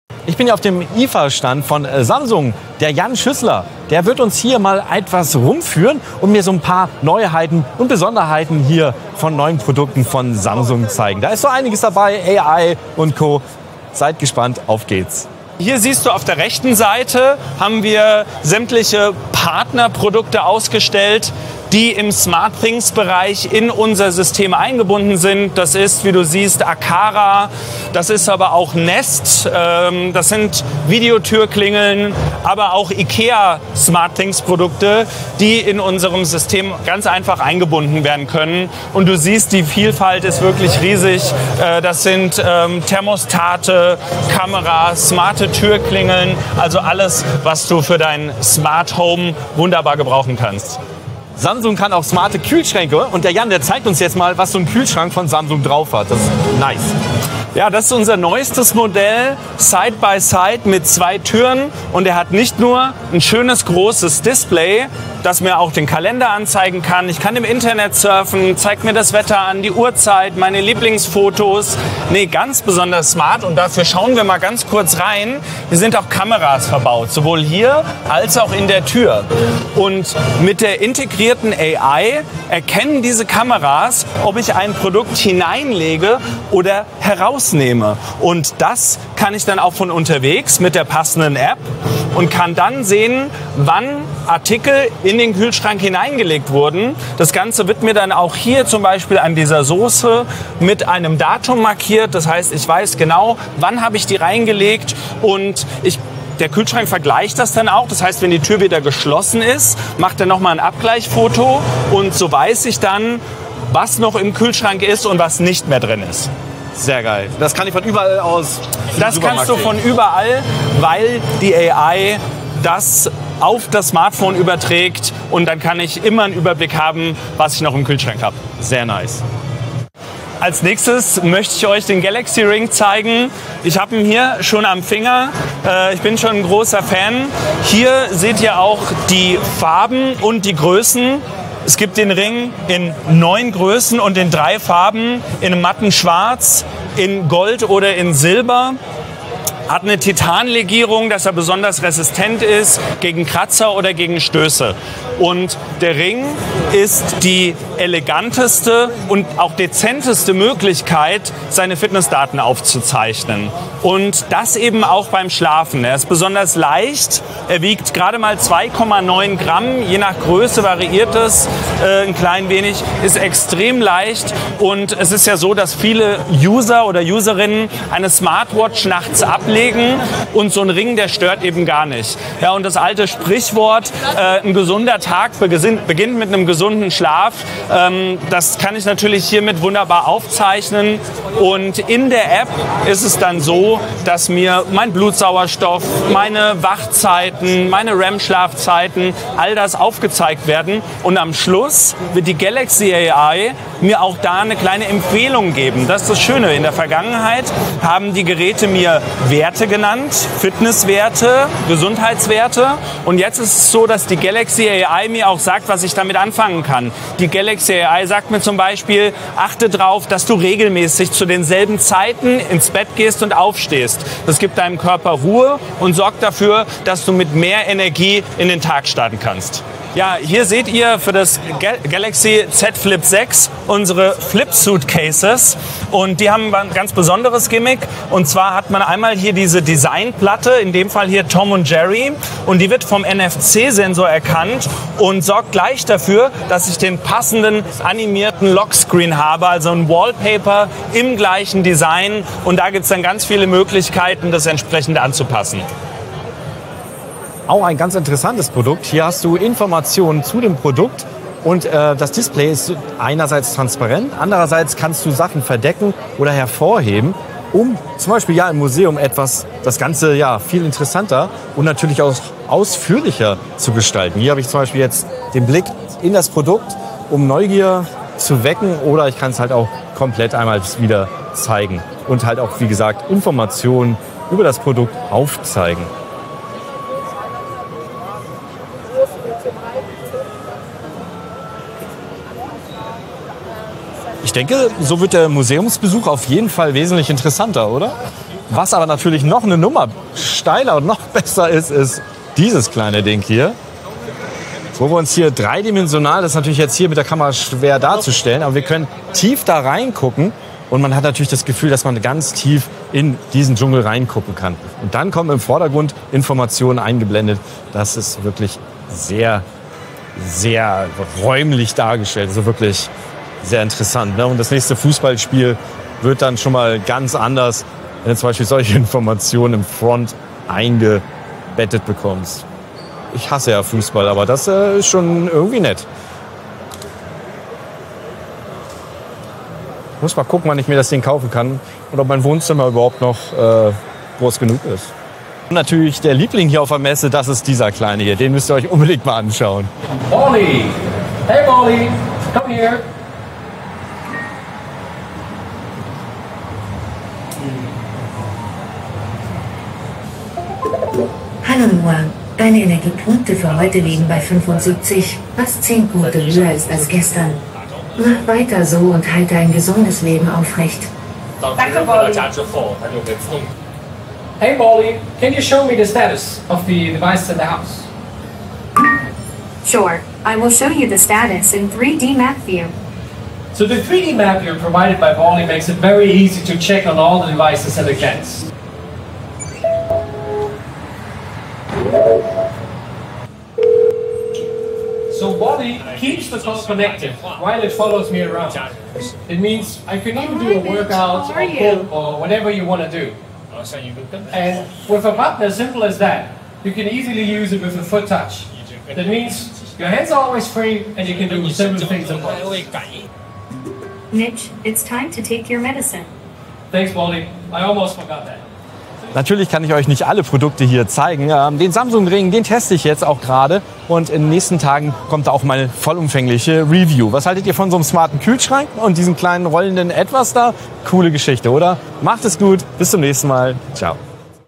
IFA 2024: Was Samsung und AI dieses Jahr zu bieten hat.
Jan Schüssler, Leiter Produktmanagement bei Samsung, nimmt uns mit auf auf dem riesigen Messestand von Samsung auf der IFA 2024. Entdecke den revolutionären KI-Kühlschrank, der deine Lebensmittel im Blick behält und dir beim Kochen hilft. Lass dich vom Galaxy Ring begeistern und lerne Ballie kennen, den niedlichen Smart-Home-Roboter, der das Leben einfacher und unterhaltsamer macht.